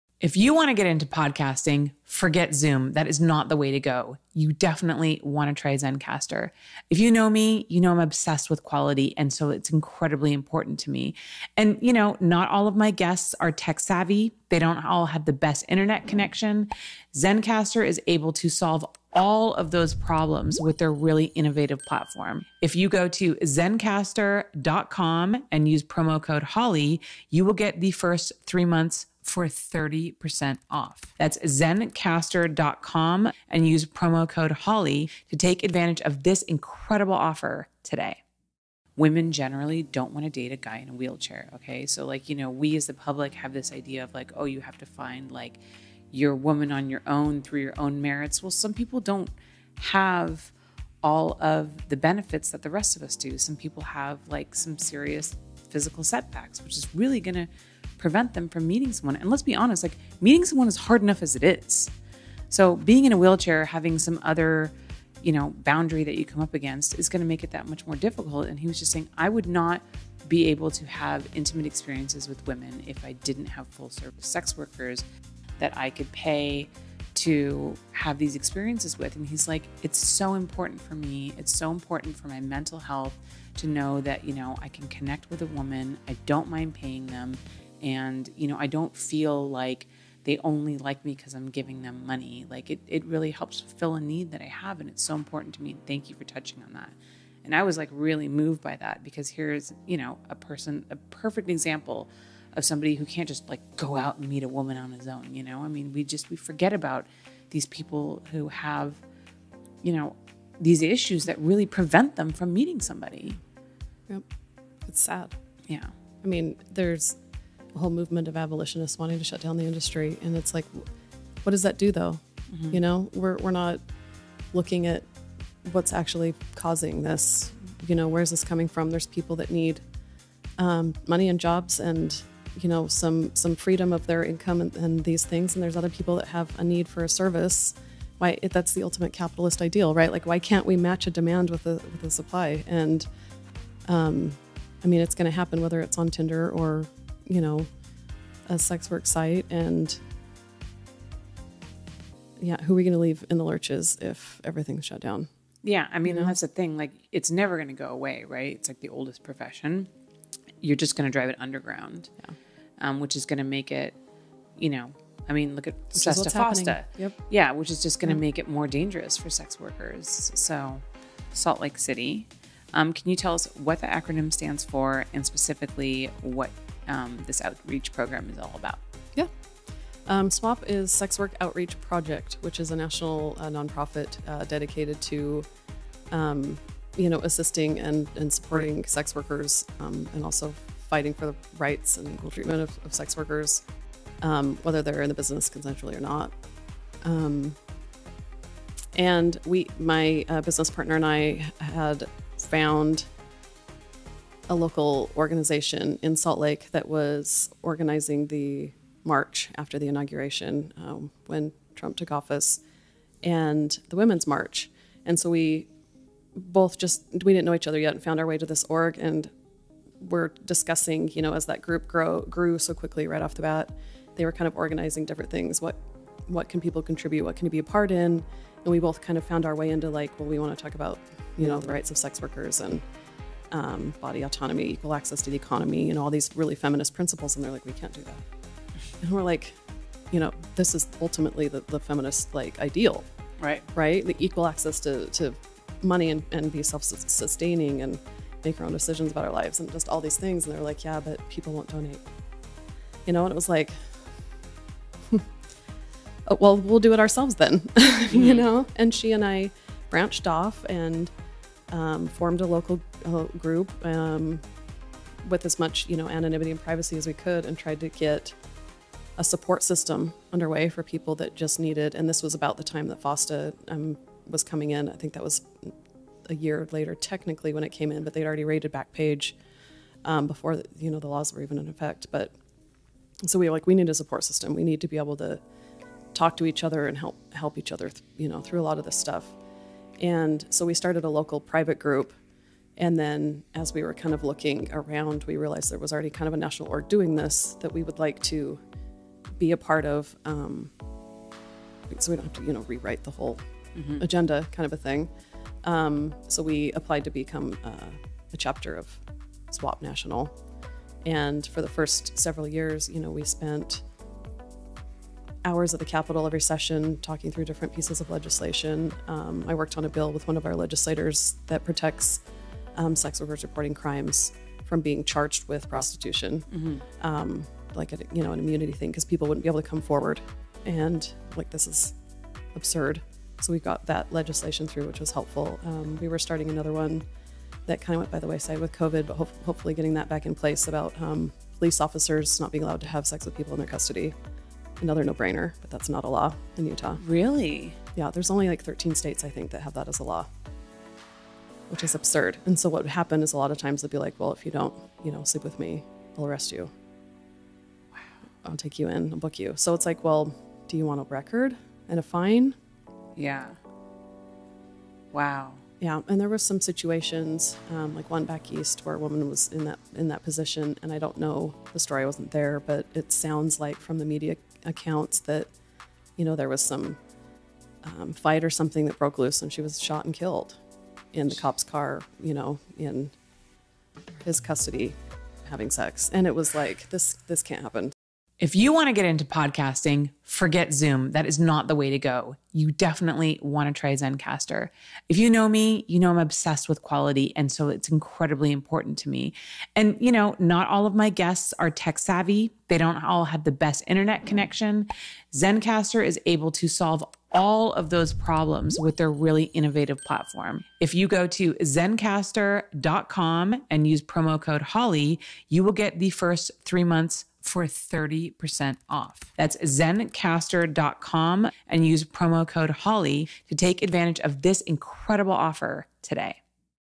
Sex Workers Deserve Rights Too
One of the most interesting points in this conversation is how Nicole partnered with SWOP to work on legislation and fight for rights and equal access for female workers in this industry.

So often sex worker's rights are neglected. There are some laws that don't exist in other states that help protect women.

In this episode, Nicole makes this point: "Why can't we match a demand with a supply?" Find out more by watching this episode and let me know what you think!